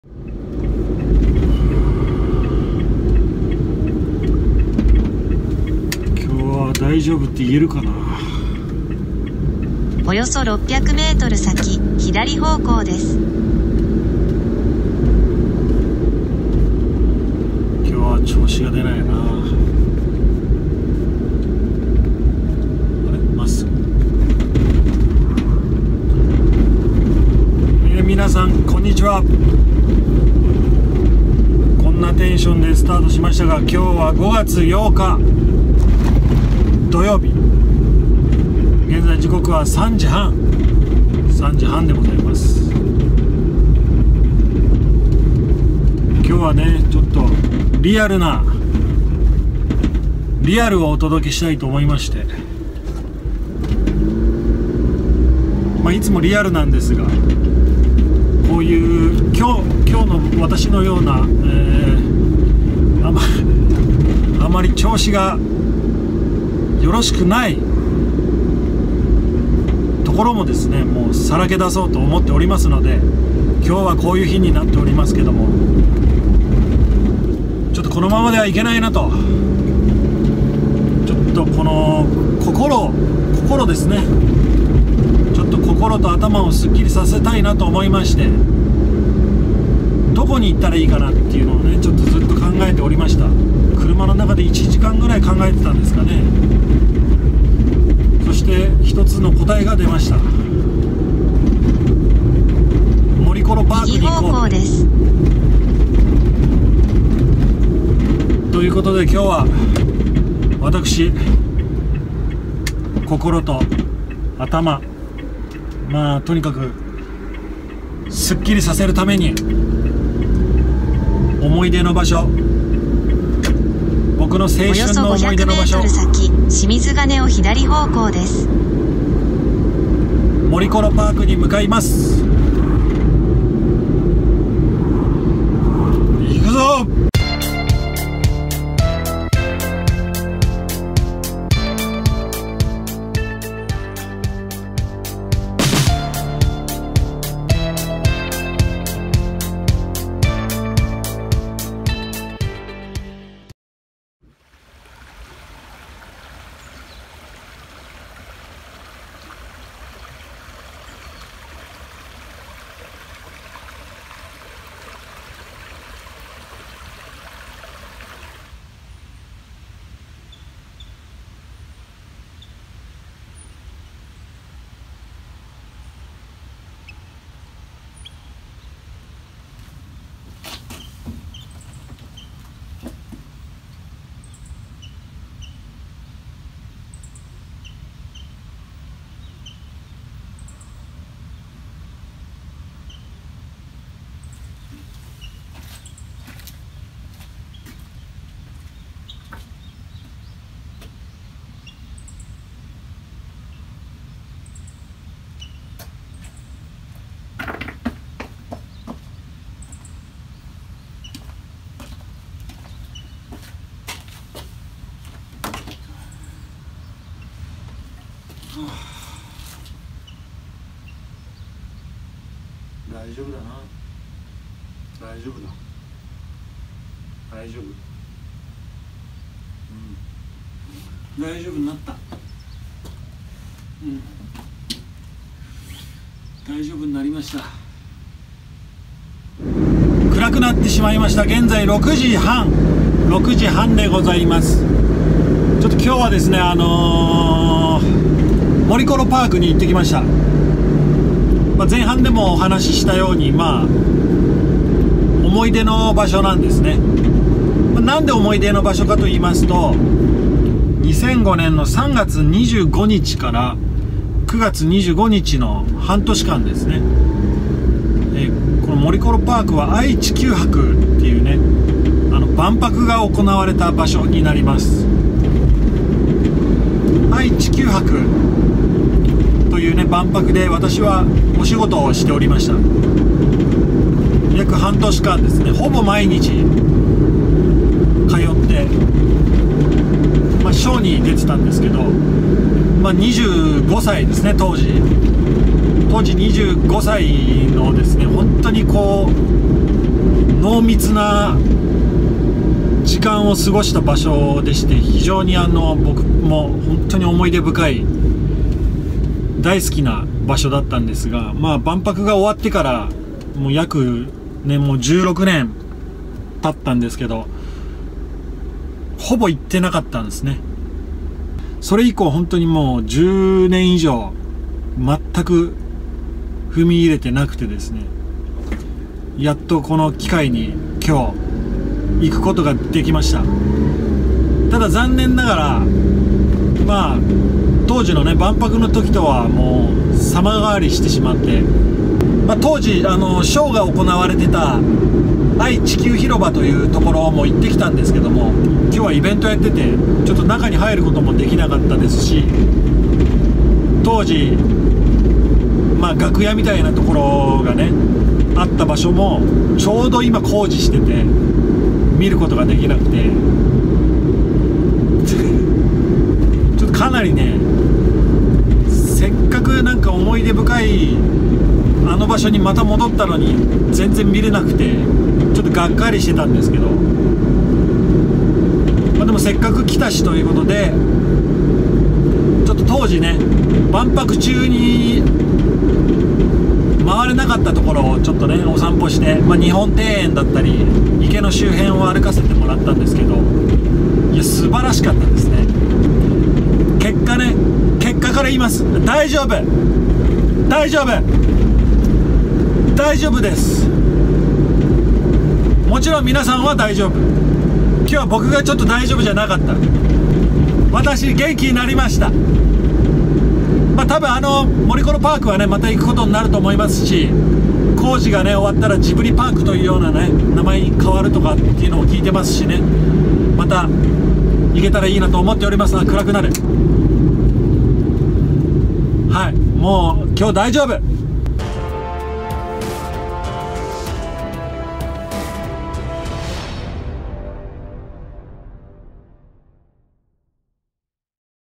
心と頭を整える為に思い出の場所へ【愛・地球博記念公園モリコロパーク】2021年5月8日(土) 大丈夫だよシリーズ
愛・地球博記念公園 モリコロパークHP